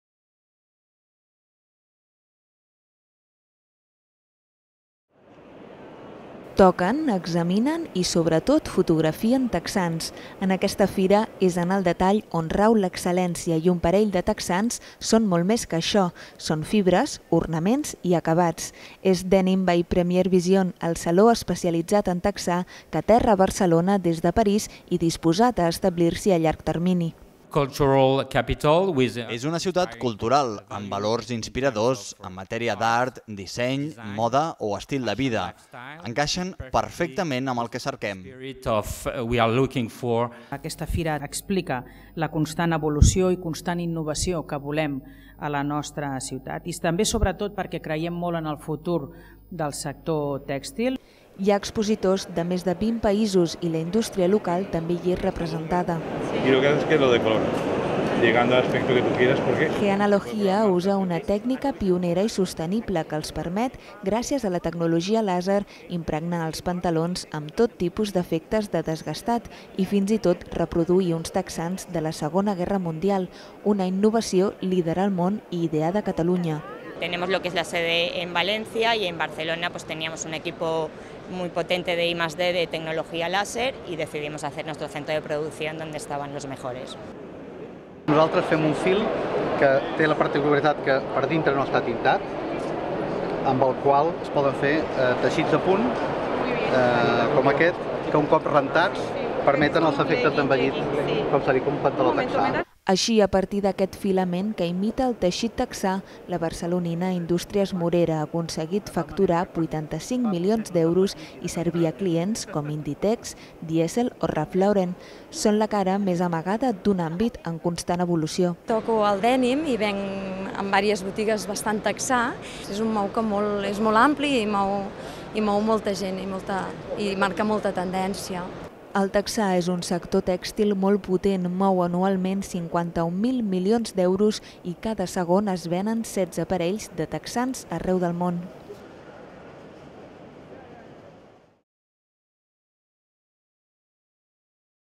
Denim by Première Vision 2014 - Més enllà del texà
El congrés Denim by Première Vision aterra a Barcelona posant de manifest la indústria que s'amaga rere els pantalons texans. El saló aplega un centenar d'expositors d'un sector que mou 51.000 milions d'euros anuals. Més informació